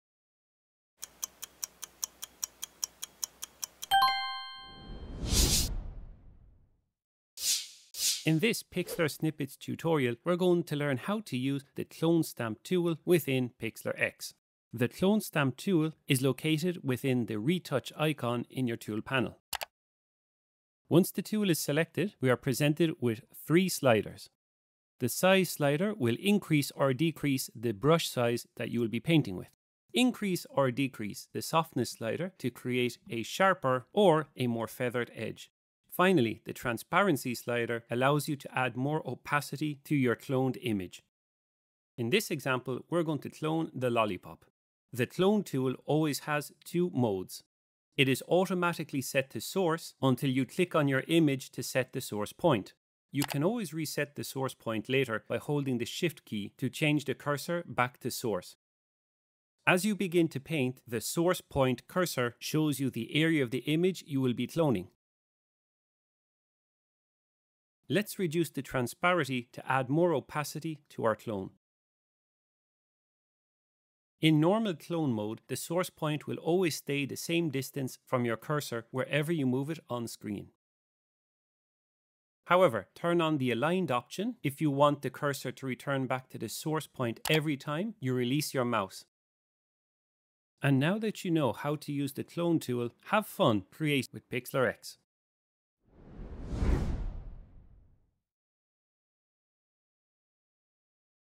Quick & Easy Series: Clone Tool in Pixlr X
In today's Quick & Easy tutorial, learn how to use the Clone tool in Pixlr X to duplicate and seamlessly blend specific parts of your image 💫

✨AI-powered online photo editor
✨Quick & Easy photo editor
✨Works on all browsers + devices